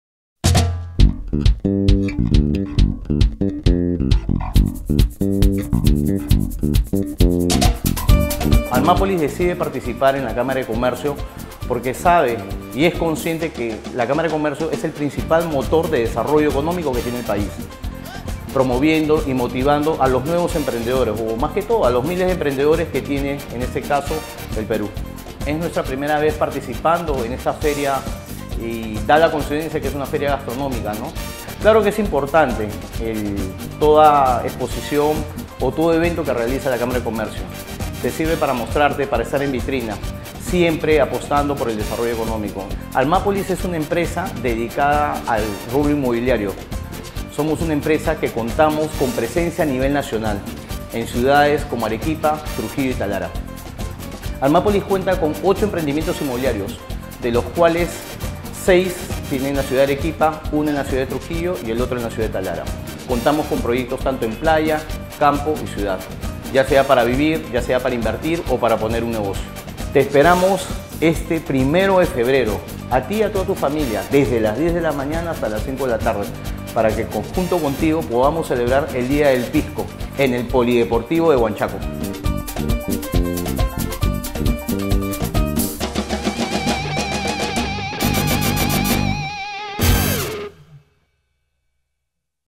17. ° Día del Pisco y 16. ° Festival Gastronómico | Almapolis
Almapolis, es una de las empresas que apuestan por el desarrollo de nuestra región. Ellos también estarán presentes en el 17. ° Día del Pisco Sour y el 16. ° Festival Gastronómico. Conoce un poco más acerca de su trabajo.